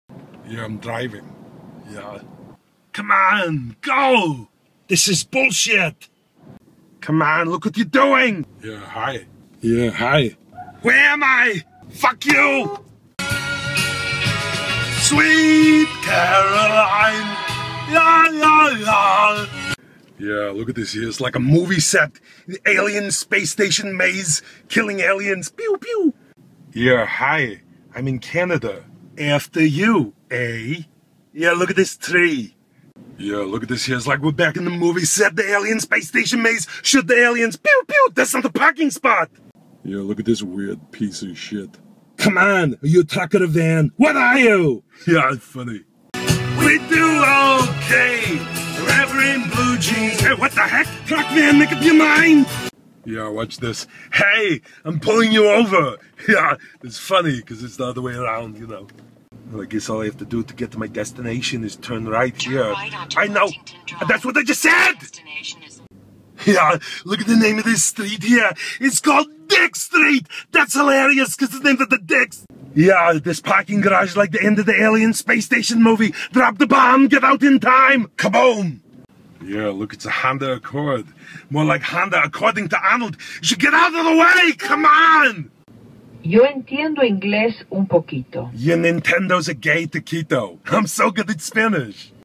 Arnold Schwarzenegger Driving by Wil Sasso (Pt. 1 - 14)
REMINDER: I AM NOT WIL SASSO!

Videos of "Arnold Schwarzenegger"'s escapades on the road.

Taken from actor Wil Sasso's Vine profile.